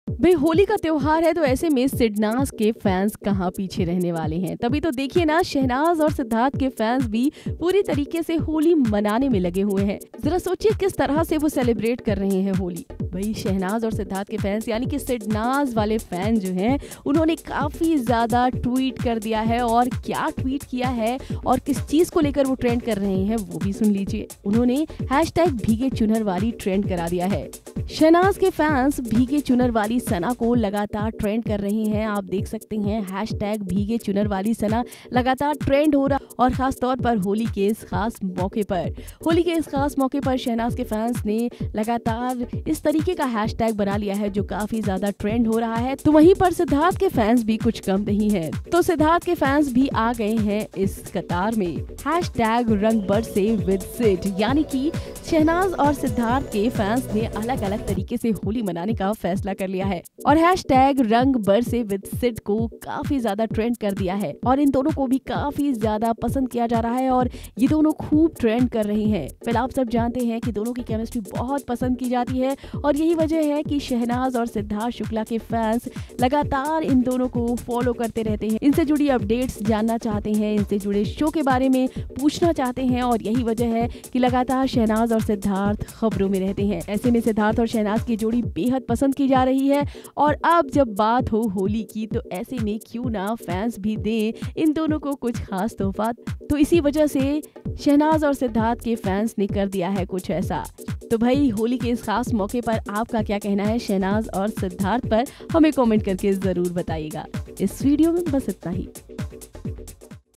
Siddharth Shehnaz फैंस पर चढ़ा होली का रंग, Trend हुआ RangBarseSid और ये | FilmiBeat
Siddharth Shukla & Shehnaz Gill's Fans special gift for Holi. Siddharth Shukla reveals on Shehnaz Gill's bond with him in Big Boss 13 house. Big Boss 13 of the most popular how Siddharth Shukla and Shahnaz age of the audience of the rich you must love.Watch video to know more.